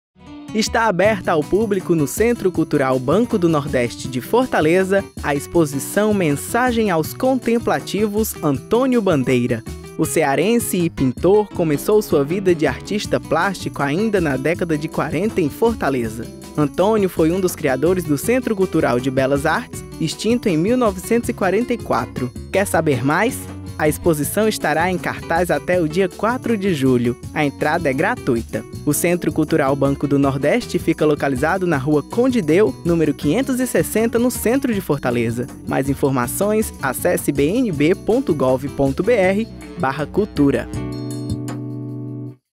Exposição "Mensagem aos Contemplativos: Antonio Bandeira"
Visitação: 27 de maio a 04 de julho de 2015.
Artista: Antônio Bandeira (CE)
Nascido em 26 de maio de 1922, o pintor cearense Antonio Bandeira começa a pintar em Fortaleza, nos primeiros anos da década de 1940 e cria, em 1941, com artistas como Mário Baratta, Raimundo Cela e Aldemir Martins, o Centro Cultural de Belas Artes-CCBA, que pretendia mobilizar a cultura visual cearense. A pintura de Antônio Bandeira nesse momento tenta figurar cenas da vida suburbana de Fortaleza sem cair nos clichês do retrato de pescadores e jangadeiros. Sua pintura segue com procedimento da abstração gestual. Ainda no Brasil, em 1962, Bandeira começa a incorporar materiais pouco usuais em suas telas, distribuindo miçangas na superfície pintada. O artista segue pintando até os dias próximos de sua morte, em 1967, em Paris.